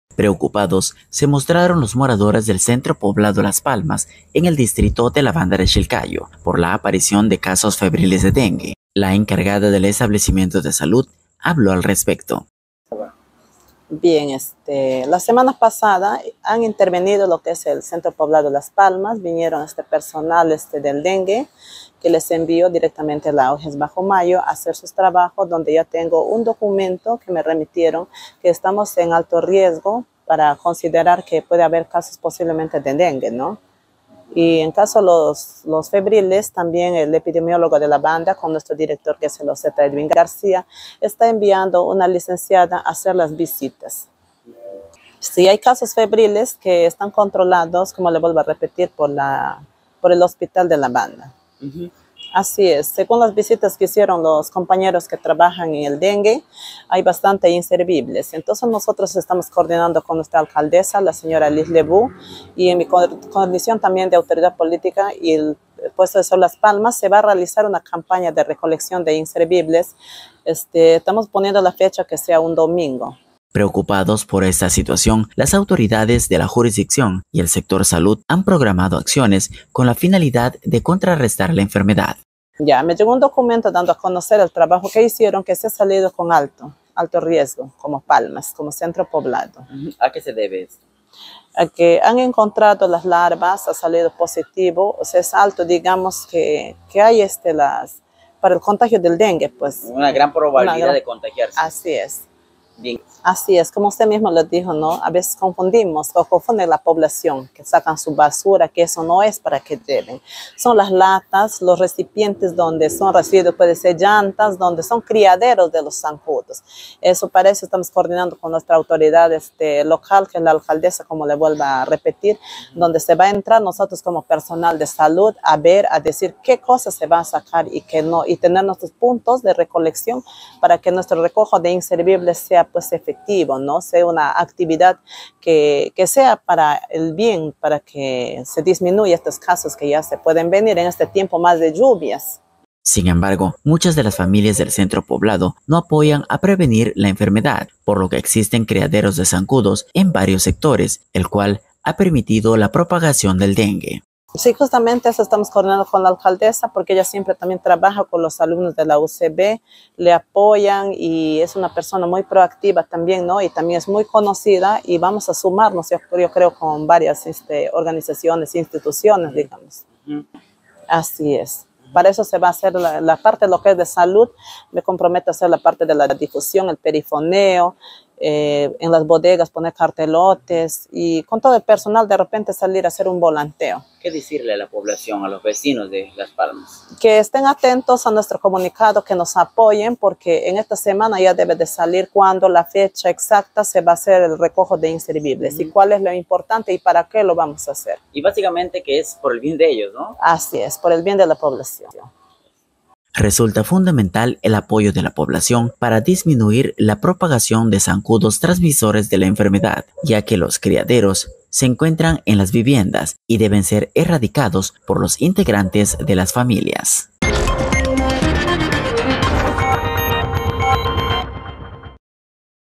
Más casos febriles de dengue aparecen en Las Palmas
Preocupados se mostraron los moradores del centro poblado Las Palmas en el distrito de La Banda de Shilcayo por la aparición de casos febriles de dengue, la encargada del establecimiento de salud habló al respecto